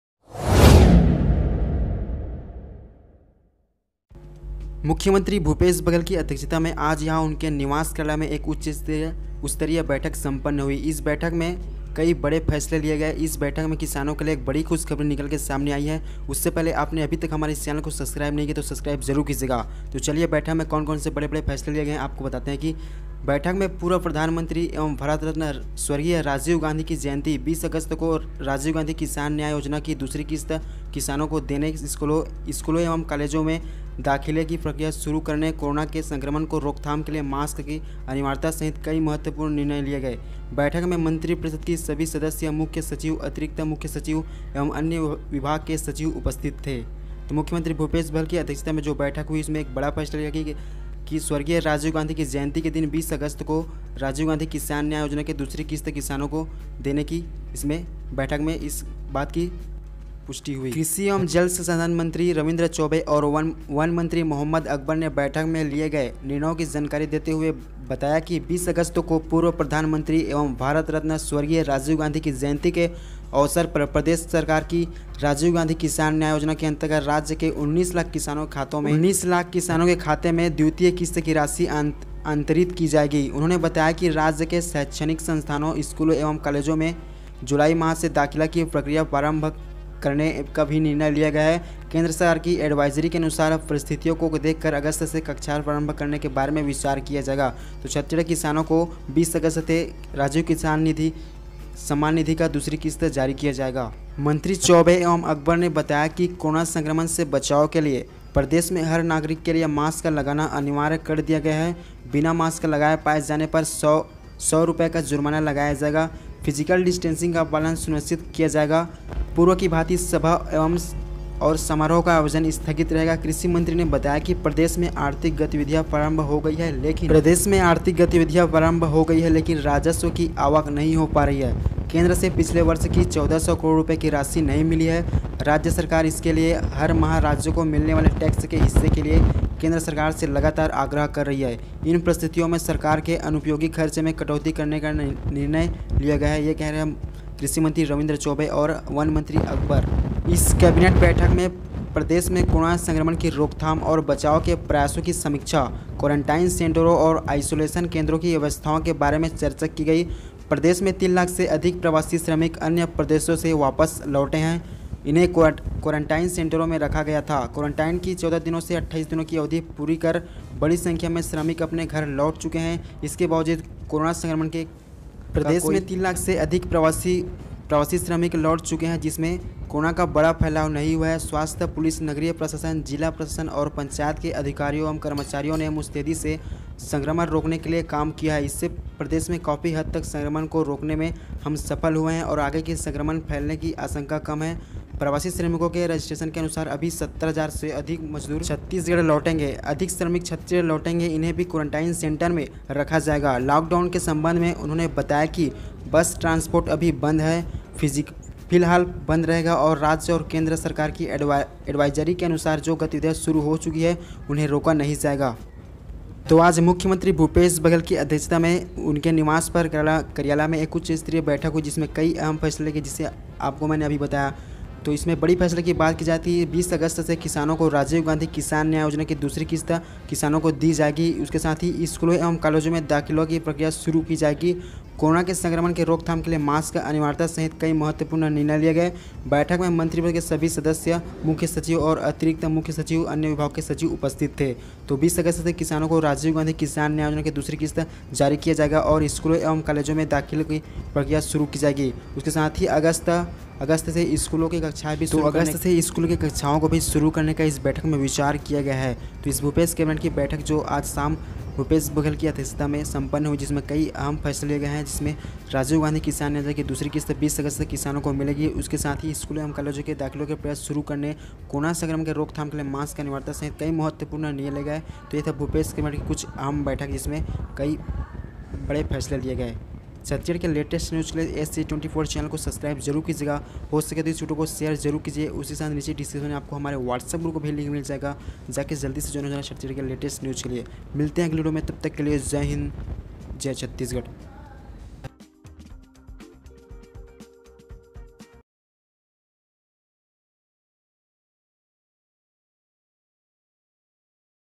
Bhupesh Baghel Cabinet Meeting Held Today in CM House Raipur |किसानों के लिए बड़ी खुशखबरी | SCG24
Thanks
scg24.in

Thanks
scg24.in jay johar

Thanks
scg24.in